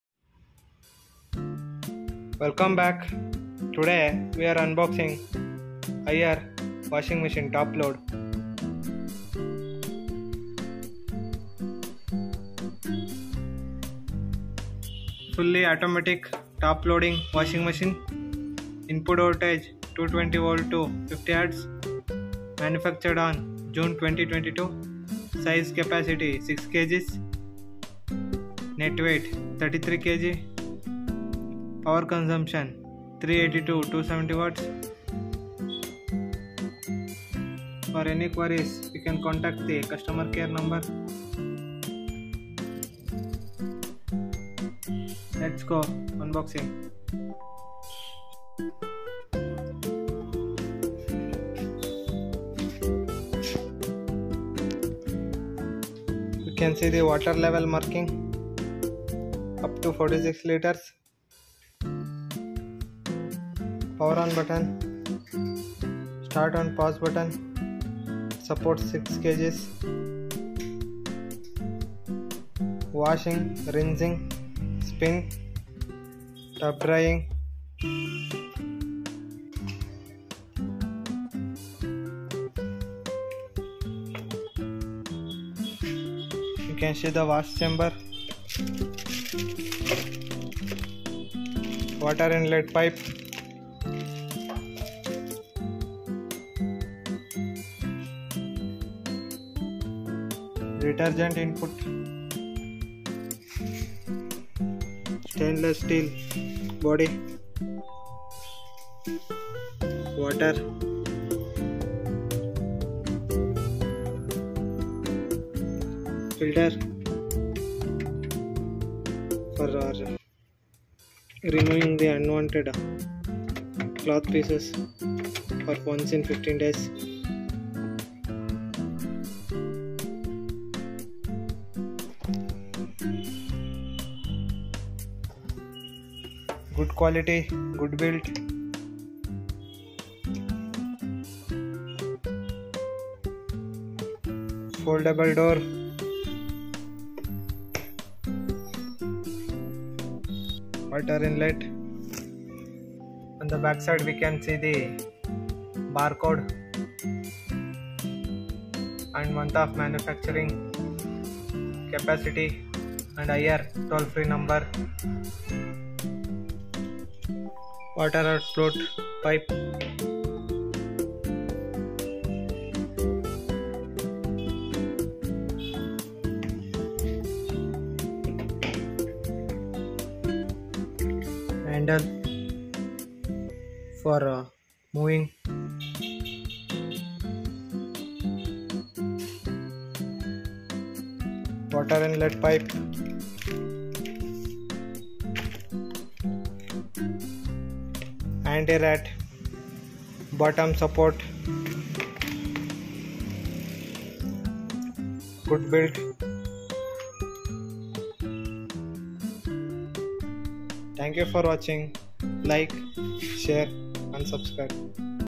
Haier Washing Machine 6KG HWM60 -1269DB
Haier washing
Oceanus Wave Drum
Huge tub with cube shape to create strong water flows, deep clean, less abrasion on clothes.

Quick Wash feature for complete cleaning in just 15 minutes.

Model Number: HWM60-1269DB
Type: Top Load Fully Automatic Washing Machine
USPs: Oceanus Wave Drum
Cabinet Color: Moonlight Grey

Spin Speed (rpm): 800
Capacity(kg): 6
Drying Capacity(kg): 6
Number of Programmes: 8
Delicate: Yes
Programmes
Delicate: Yes
Quick Wash: Yes
Ariel Wash: Yes
Wash: Yes
Tub Dry: Yes
Rinse: Yes
Spin: Yes
Soak: Yes
Options
Quick Wash1: Yes
Drain: Yes
Functions
Display: LED
Timer: Yes
Child Lock: Yes
Equipment
Fuzzy Logic: Yes
Energy
Voltage / Frequency: 220/50
Dimensions
Net Weight / Gross Weight (kg): 520*540*930